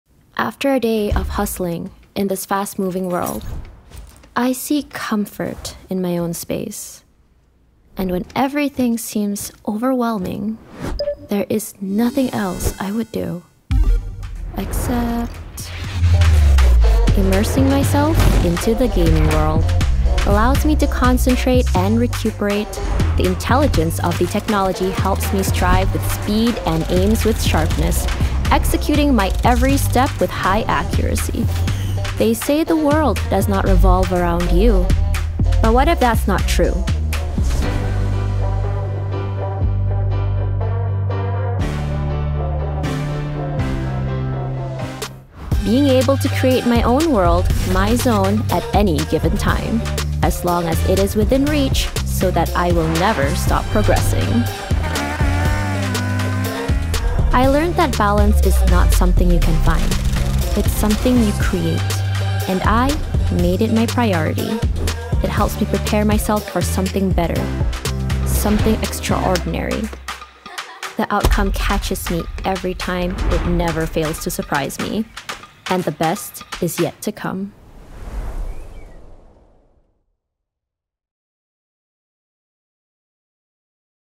Nitro 5 - The Journey Lies Within
Built for the speedy, immerse yourself in gaming with Acer Nitro devices. Sharpen your senses with dedicated technology dedicated for enhanced performance and health.

Precision and control no matter the occasion. Go full throttle with the Nitro, more